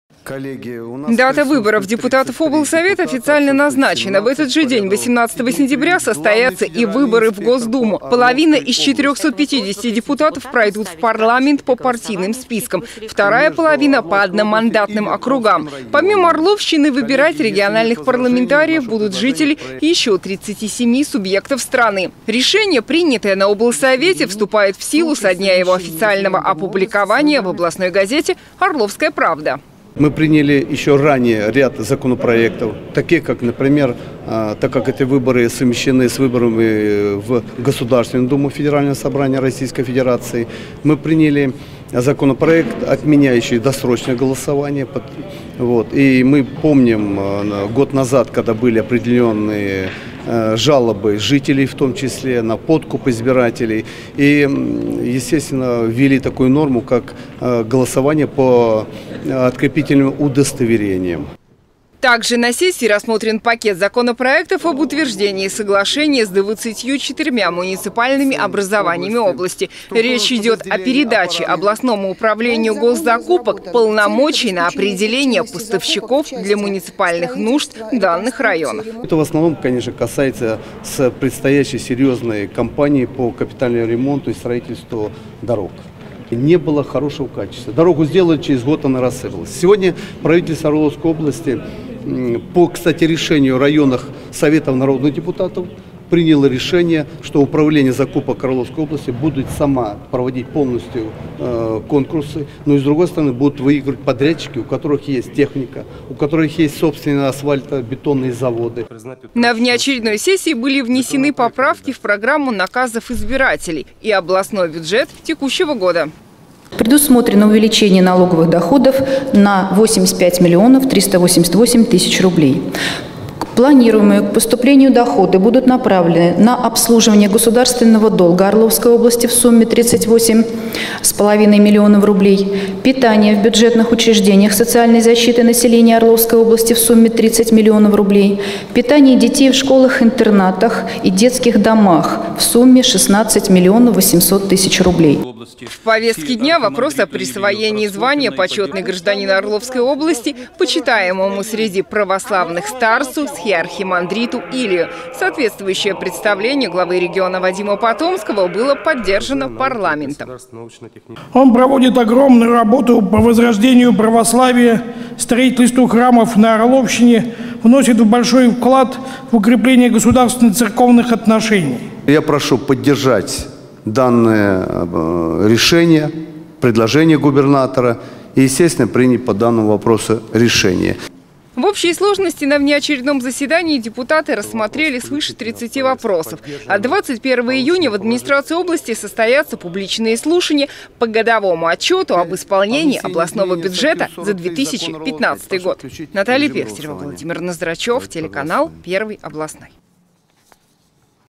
Сессия Областного Совета
Соответствующее представление главы региона Вадима Потомского сегодня было поддержано на внеочередном заседании Орловского областного Совета народных депутатов.

Как отметил первый заместитель Губернатора и Председателя Правительства Орловской области Александр Бударин, уроженец Орловщины схиархимандрит Илий в православной среде считается одним из самых авторитетных и известных духовников нашего времени.

Он проводит огромную работу по укреплению нравственности, строительству храмов, восстановлению исторического облика региона